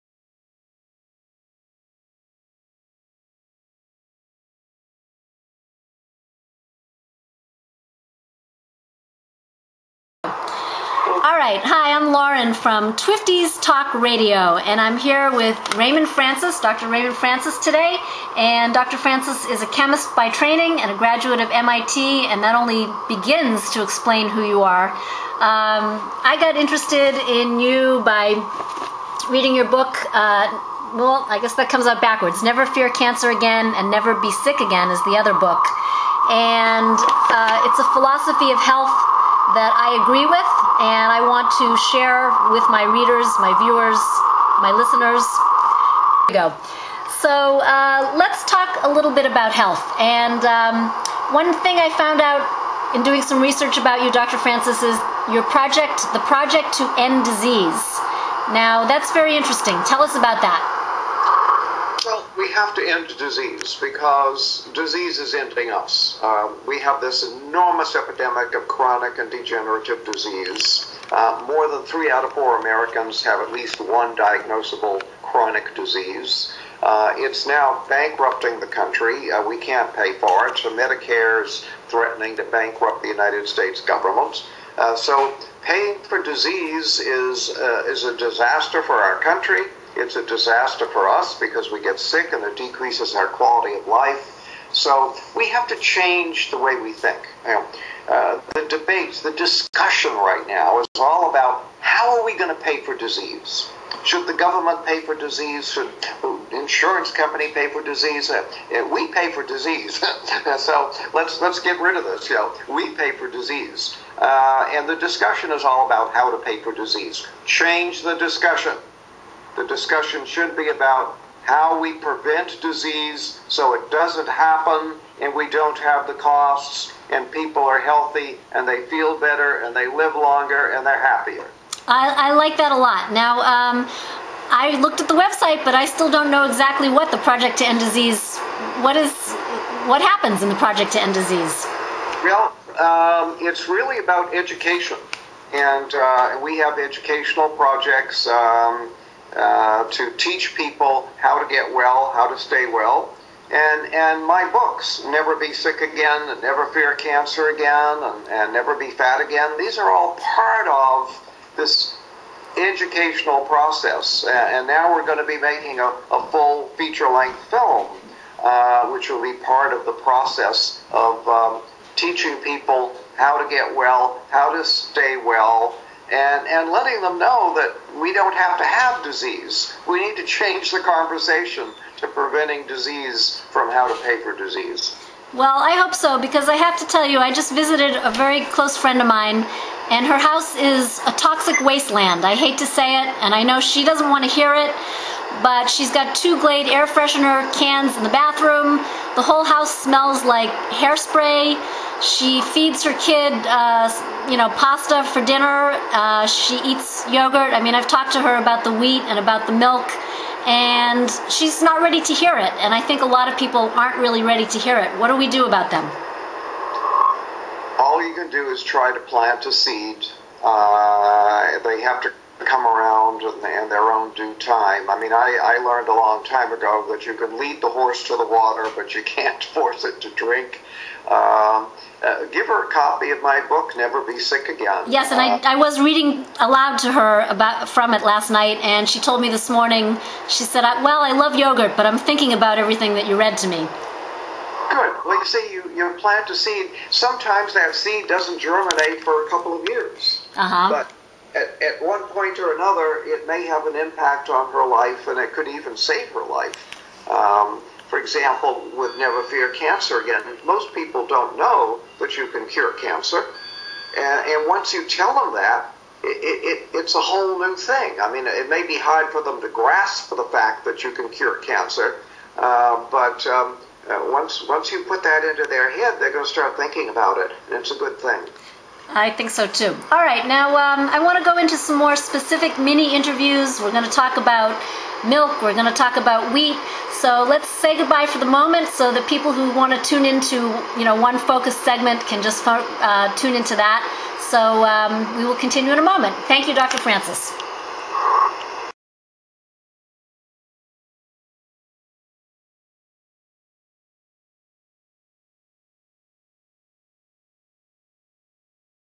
We Can End Disease - Raymond Francis Interview
Raymond Francis,an MIT trained chemist, author, health visionary and the founder of The Project to End Disease wants to help people create and preserve health.
He states "more than 3 out of 4 Americans have at least one diagnosable chronic disease. Disease decreases the quality of our life. Who is going to pay for disease? Change the discussion. The discussion should be 'How can we prevent disease?' Most people don't know that you can cure cancer, and once you tell them that, it's a whole new thing."

Raymond Francis, M.Sc. is the author of "Never Be Sick Again,"Never Fear Cancer Again," and "Never Be Fat Again" and is the founder of The Project to End Disease - TPED.org.

Watch or listen to this interview of Raymond Francis, by Lauren Traub Teton
Then check out Twifties are the fun (and healthy!) people around age 50 and up!